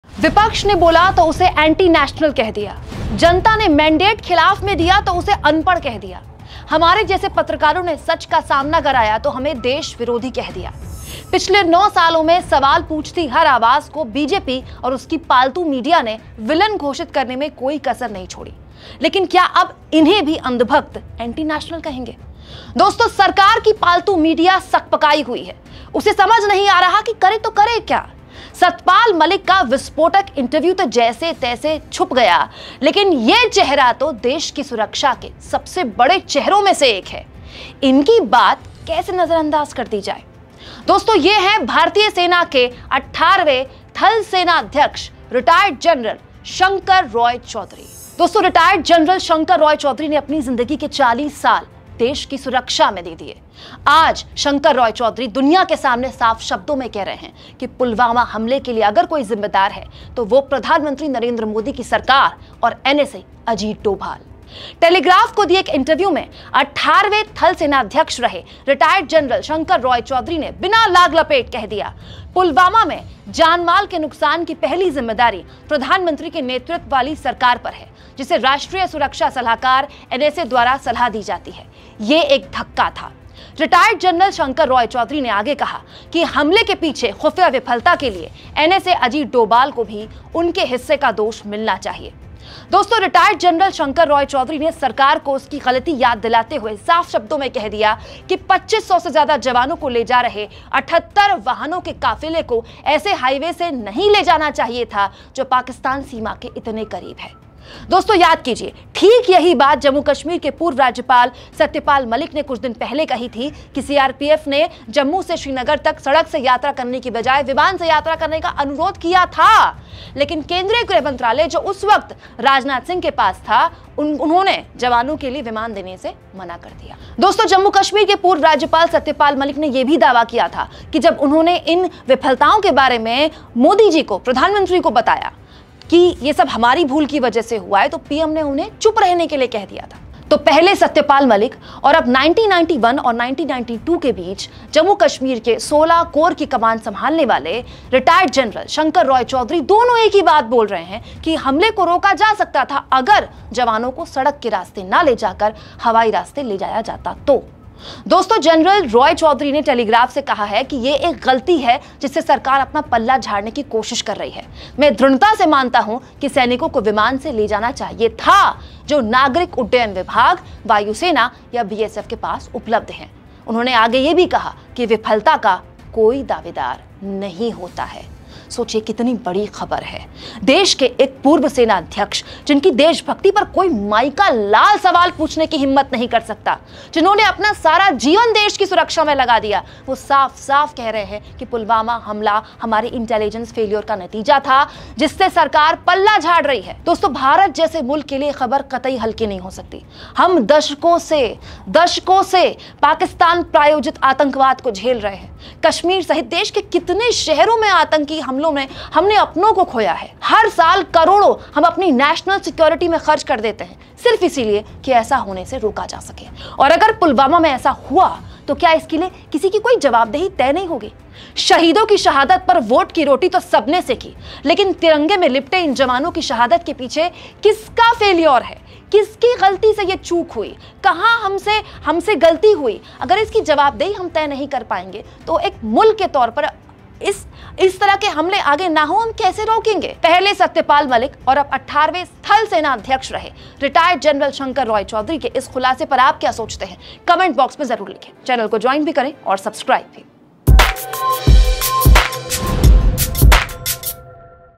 PM Modi - NSA Ajit Doval पर उठाए सवाल! अब EX Army Chief Shankar Roychowdhury Pulwama पर क्या बोले?
Former Army chief Shankar Roychowdhury raised questions on PM Modi and NSA Ajit Doval in an interview given to the Telegraph Newspaper. Shankar Roy Chowdhury said that PM Modi and Ajit Doval should take responsibility for Pulwama. Analysis of Kumkum Binwal's of Shankar Roy Chowdhury's interview given to the Telegraph.